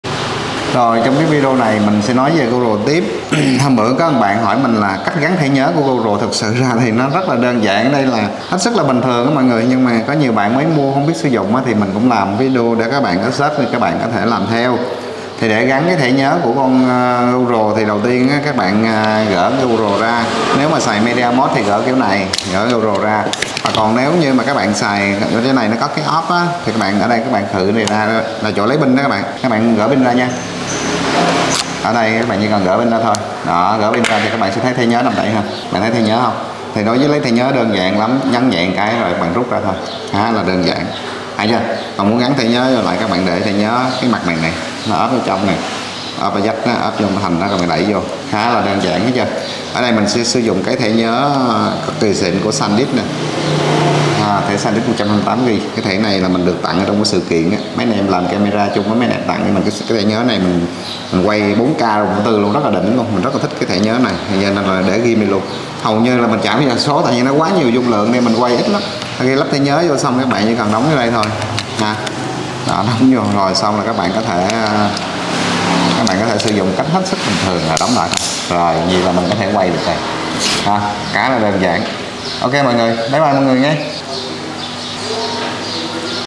Cách tháo lắp thể nhớ và media mod gopro
Bữa có bạn hỏi cách lắp đặt thẻ nhớ cho gopro và media mod nên mình làm cái clip này để hướng dẫn bạn cách xử lý nhé.
CAMERA PHAN THIẾT - Cung cấp giải pháp camera an ninh, Giải pháp mạng, Nhà thông minh.
Địa chỉ: 07 Nguyễn Tương - Phan Thiết - Bình Thuận